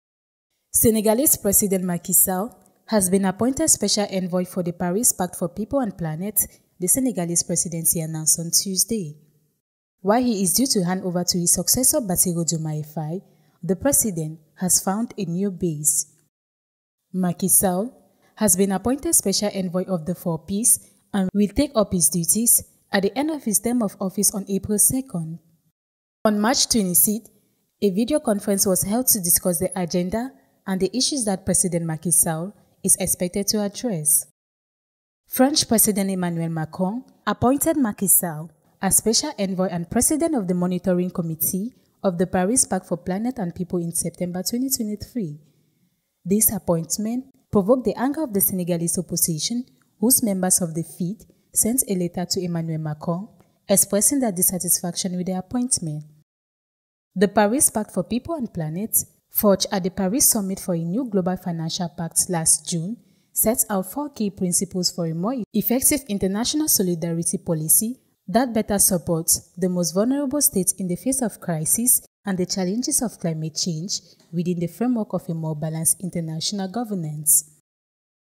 SENEGAL MACKY SALL APPOINTED SPECIAL ENVOY FOR THE 4PS PARIS PACT FOR PEOPLE AND PLANET
Senegalese President Macky Sall has been appointed Special Envoy for the 4Ps (Paris Pact for People and Planet), the Senegalese Presidency announced on Tuesday.  While he is due to hand over to his successor Bassirou Diomaye Diakhar Faye on 02 April, the President of the Republic, Macky Sall, has found a new base.